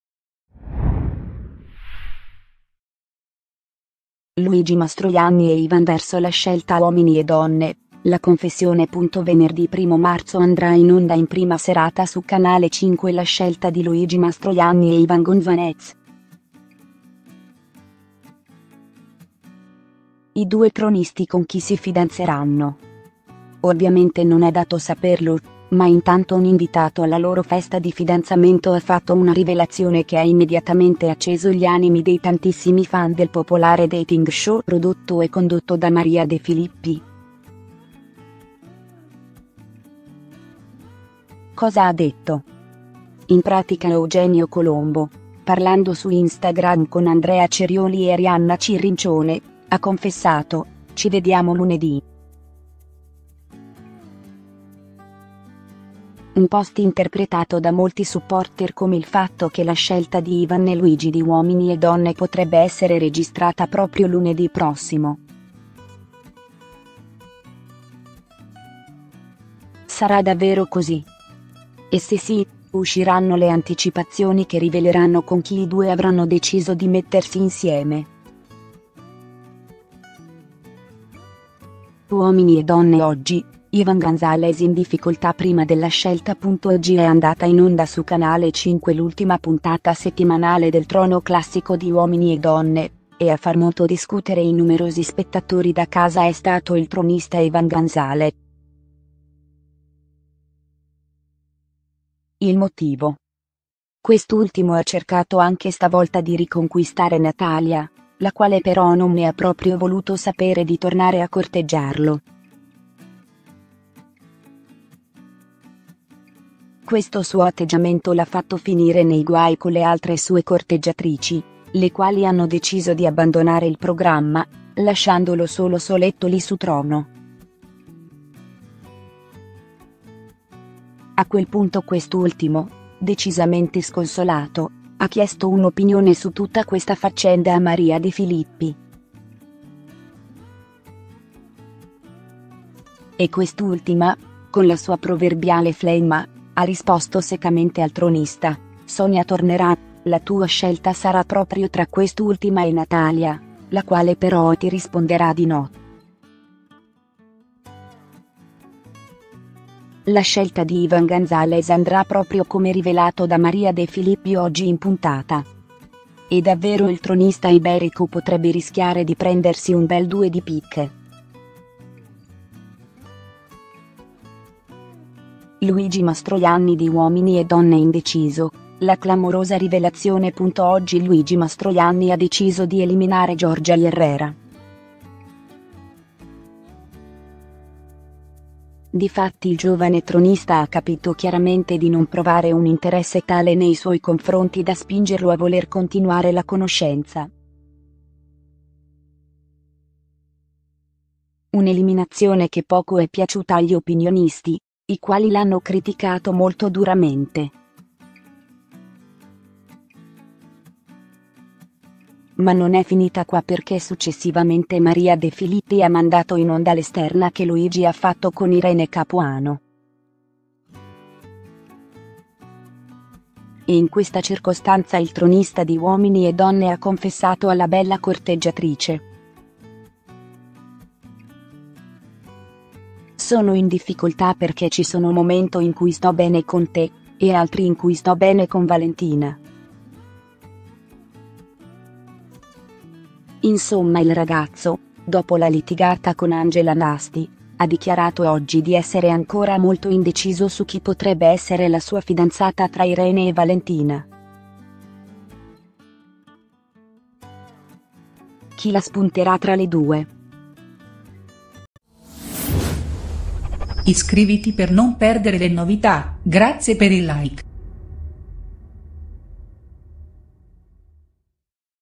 Anticipazioni UeD, scelta Luigi e Ivan: invitato fa una rivelazione
Luigi Mastroianni e Ivan verso la scelta a Uomini e Donne: la confessione
Venerdì 1 Marzo andrà in onda in prima serata su Canale 5 la scelta di Luigi Mastroianni e Ivan Gonzaez. I due tronisti con chi si fidanzeranno? Ovviamente non è dato saperlo, ma intanto un invitato alla loro festa di fidanzamento ha fatto una rivelazione che ha immediatamente acceso gli animi dei tantissimi fan del popolare dating show prodotto e condotto da Maria De Filippi. Cosa ha detto? In pratica Eugenio Colombo, parlando su instagram con Andrea Cerioli e Arianna Cirrincione, ha confessato: “Ci vediamo Lunedì”. Un post interpretato da molti supporter come il fatto che la scelta di Ivan e Luigi di Uomini e Donne potrebbe essere registrata proprio Lunedì prossimo. Sarà davvero così? E se sì, usciranno le anticipazioni che riveleranno con chi i due avranno deciso di mettersi insieme?

Uomini e Donne oggi: Ivan Gonzalez in difficoltà prima della scelta

Oggi è andata in onda su Canale 5 l’ultima puntata settimanale del Trono Classico di Uomini e Donne, e a far molto discutere i numerosi spettatori da casa è stato il tronista Ivan Gonzalez. Il motivo? Quest’ultimo ha cercato anche stavolta di riconquistare Natalia, la quale però non ne ha proprio voluto sapere di tornare a corteggiarlo. Questo suo atteggiamento l’ha fatto finire nei guai con le altre sue corteggiatrici, le quali hanno deciso di abbandonare il programma, lasciandolo solo soletto lì su trono. A quel punto quest’ultimo, decisamente sconsolato, ha chiesto un’opinione su tutta questa faccenda a Maria De Filippi. E quest’ultima, con la sua proverbiale flemma, ha risposto seccamente al tronista: “Sonia tornerà…La tua scelta sarà proprio tra quest’ultima e Natalia, la quale però ti risponderà di no..” La scelta di Ivan Gonzalez andrà proprio come rivelato da Maria De Filippi oggi in puntata? E davvero il tronista iberico potrebbe rischiare di prendersi un bel due di picche?

Luigi Mastroianni di Uomini e Donne indeciso: la clamorosa rivelazione

Oggi Luigi Mastroianni ha deciso di eliminare Giorgia Iarrera. Difatti il giovane tronista ha capito chiaramente di non provare un interesse tale nei suoi confronti da spingerlo a voler continuare la conoscenza. Un’eliminazione che poco è piaciuta agli opinionisti, i quali l’hanno criticato molto duramente. Ma non è finita qua perchè successivamente Maria De Filippi ha mandato in onda l’esterna che Luigi ha fatto con Irene Capuano. E in questa circostanza il tronista di Uomini e Donne ha confessato alla bella corteggiatrice:

“Sono in difficoltà perchè ci sono momento in cui sto bene con te, e altri in cui sto bene con Valentina…”

Insomma il ragazzo, dopo la litigata con Angela Nasti, ha dichiarato oggi di essere ancora molto indeciso su chi potrebbe essere la sua fidanzata tra Irene e Valentina. Chi la spunterà tra le due?